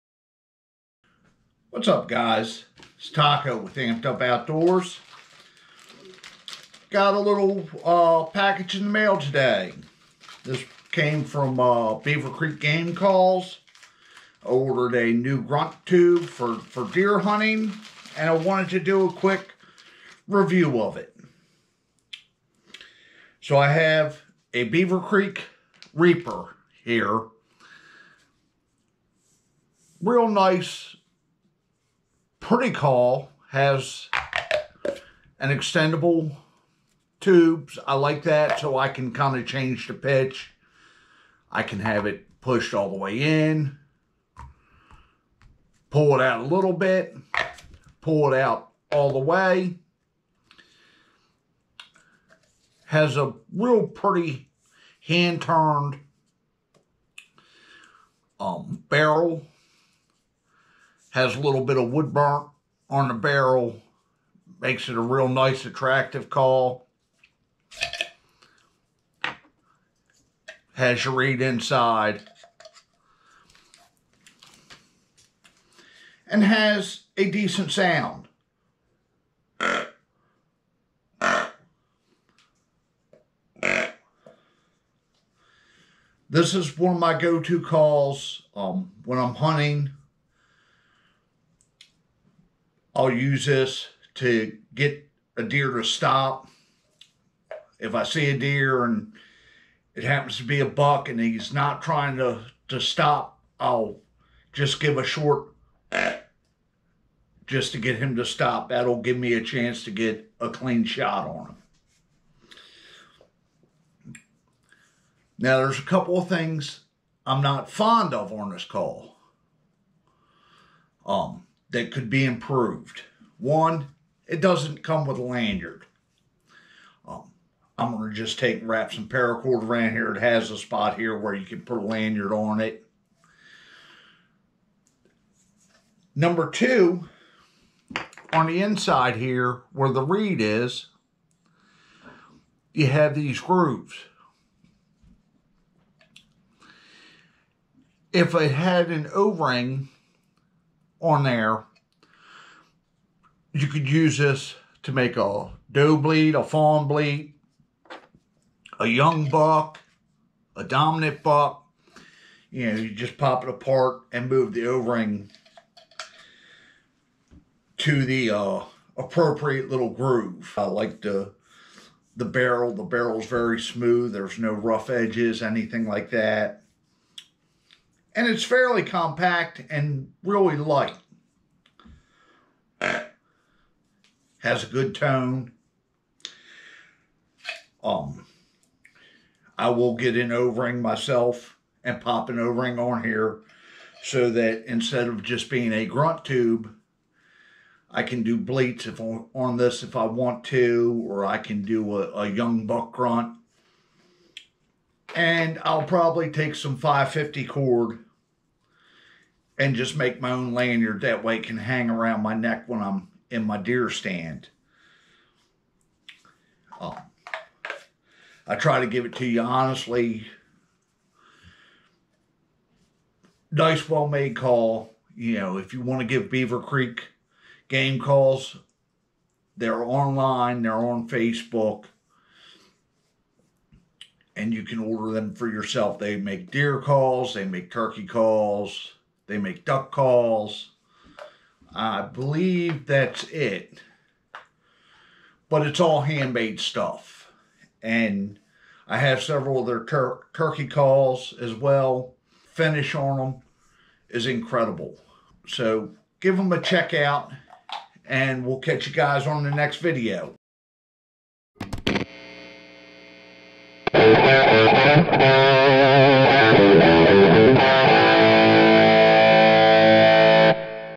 Beaver Creek Grunt Tube review
Taco gives a quick review of Beaver Creek's Grunt Tube for calling in those white tail deer,